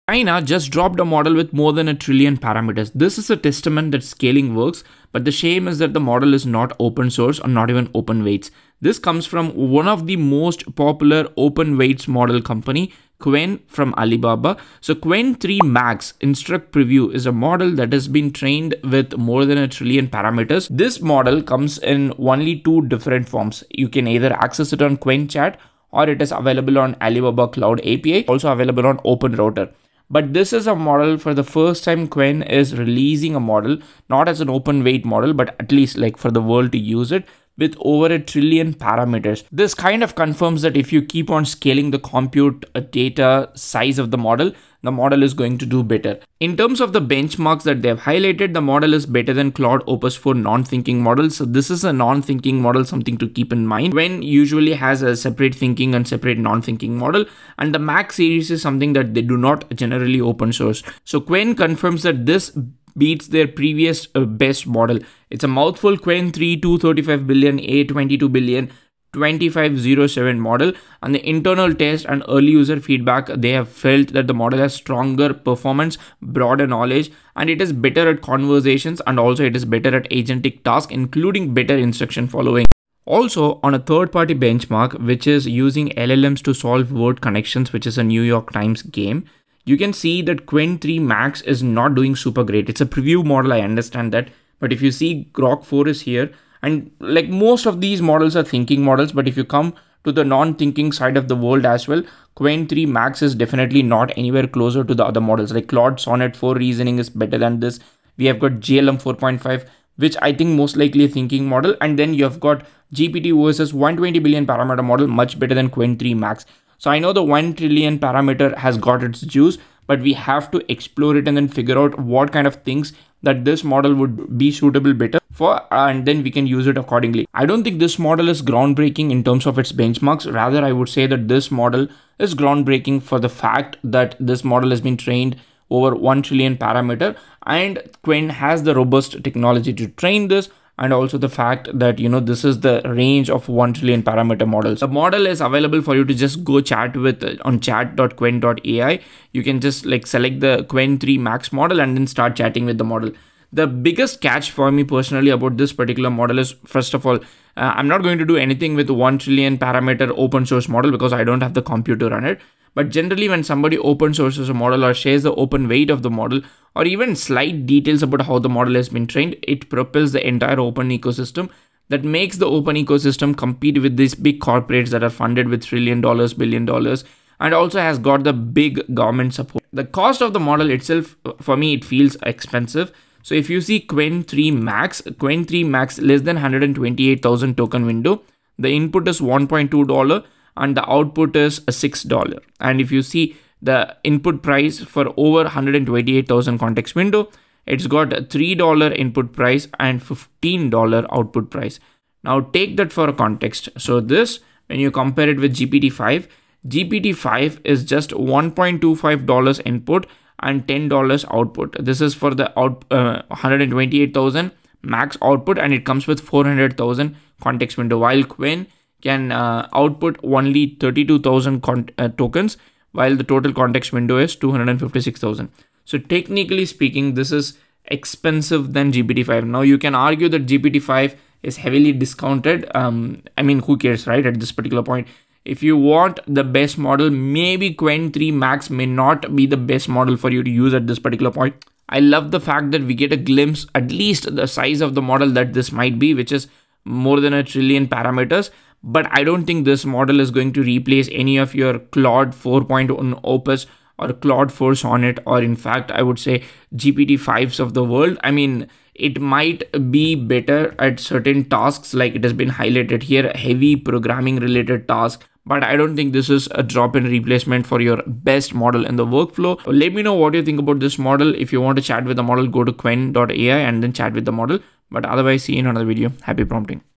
NEW: China's 1+ Trillion parameter model!!!
Introducing Qwen3-Max-Preview (Instruct) — our biggest model yet, with over 1 trillion parameters! 🚀

Now available via Qwen Chat & Alibaba Cloud API.

Benchmarks show it beats our previous best, Qwen3-235B-A22B-2507. Internal tests + early user feedback confirm: stronger performance, broader knowledge, better at conversations, agentic tasks & instruction following.